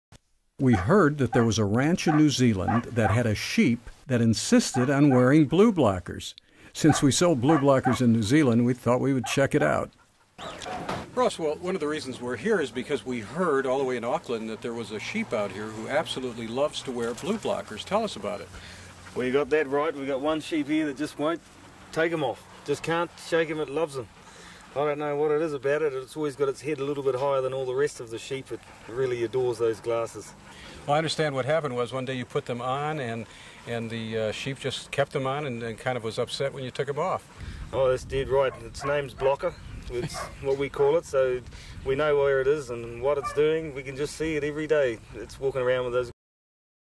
BluBlocker - Sheep wears sunglasses
A sheep in New Zealand can't live without BluBlockers. You can't make this stuff up.